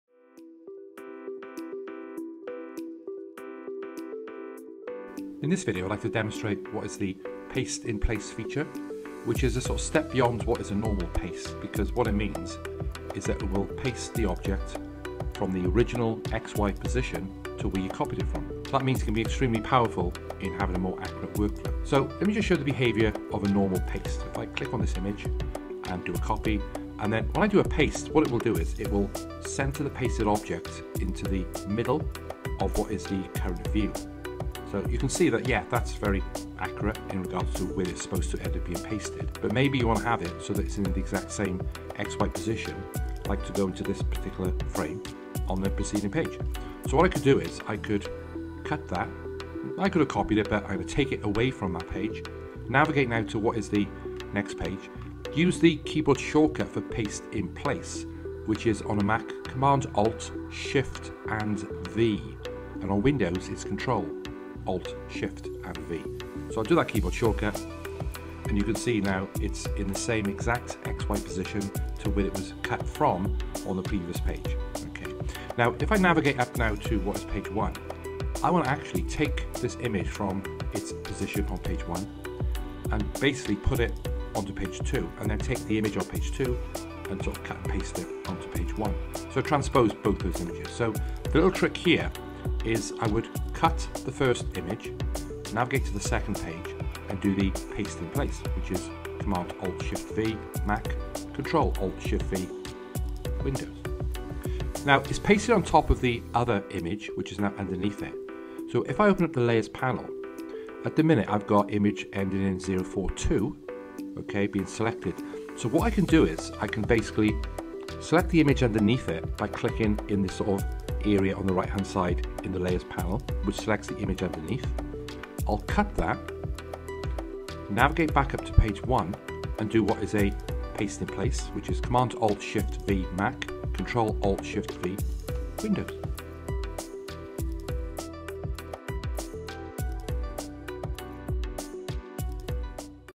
How do I copy and paste into the same position in InDesign?
We show in this video how to enhance the standard paste by using paste-in-place. This powerful feature will paste the copied object to the same X and Y position to where it was originals copied from.

For those looking to learn more about InDesign, we have a range of Adobe authorised training courses available